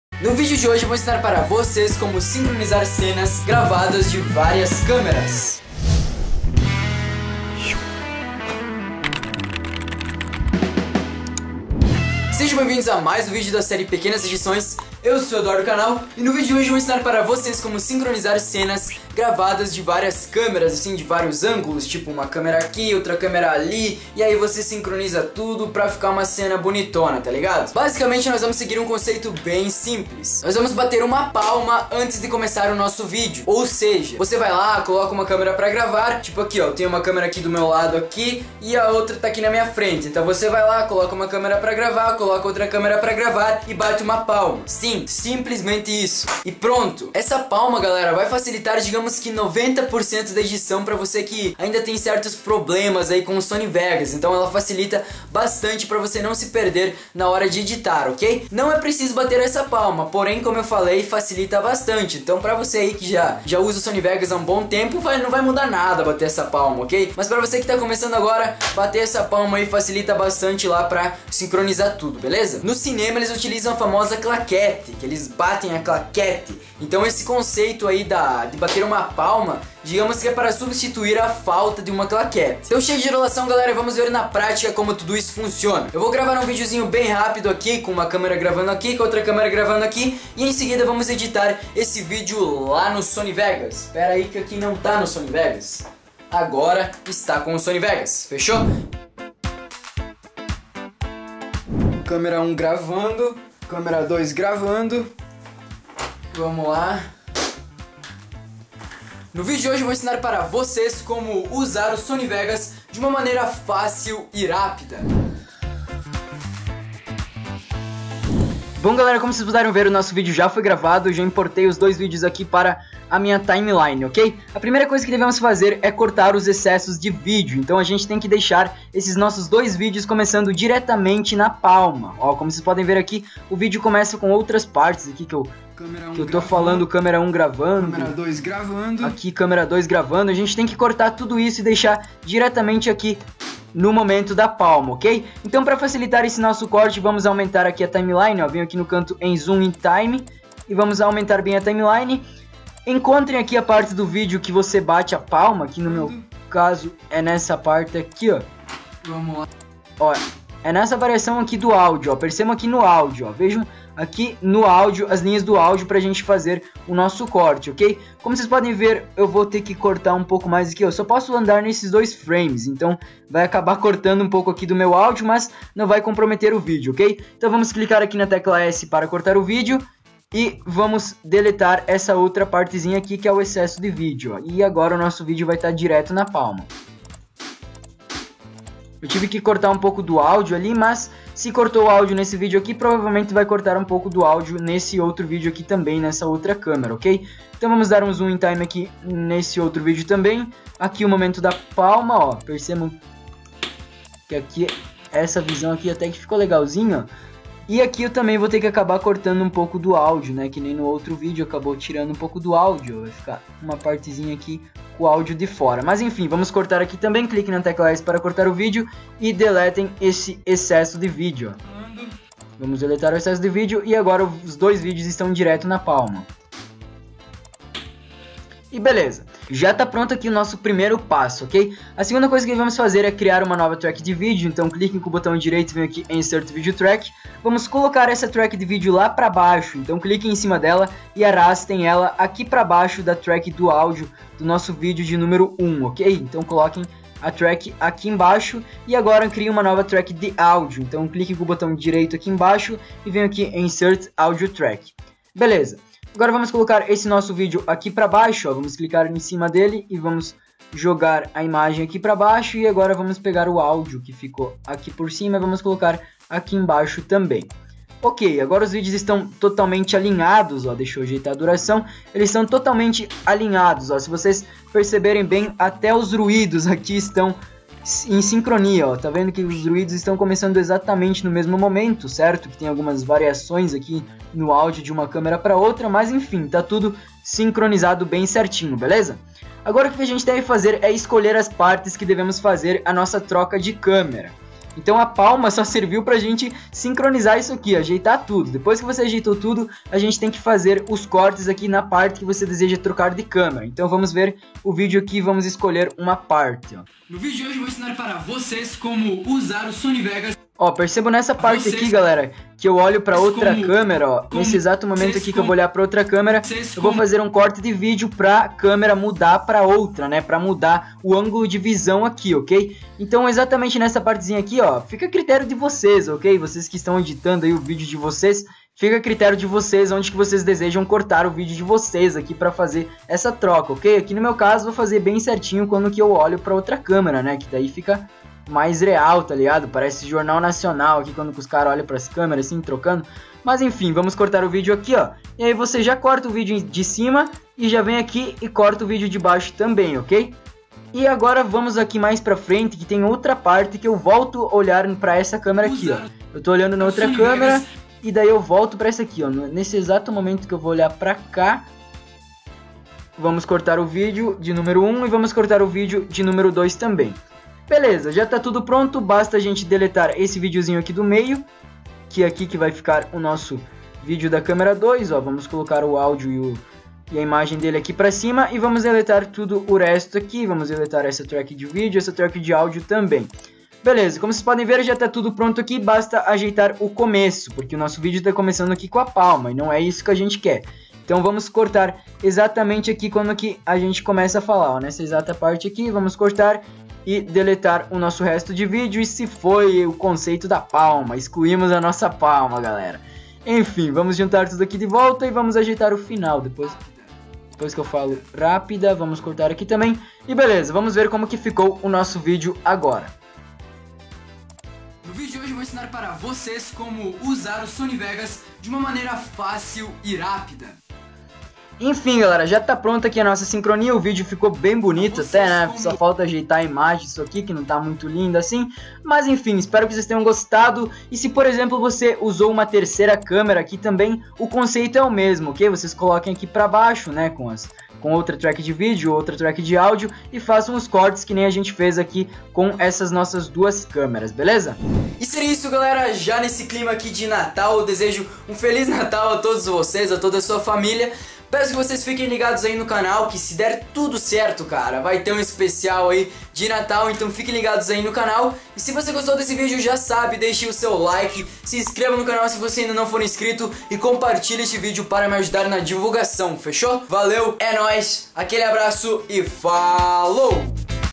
Sincronizar Cenas - Tutorial Sony Vegas
Assista ao vídeo e aprenda como SINCRONIZAR CENAS no Sony Vegas!

Dicas sobre a Gravação: 0:27
Gravação das Cenas: 1:39
Sincronizando as Gravações: 1:55

Acompanhe a série Pequenas Edições e aprenda algumas coisas básicas para editar seus vídeos:

Tem alguma dúvida ou sugestão? Basta enviar seu comentário que respondo assim que possível!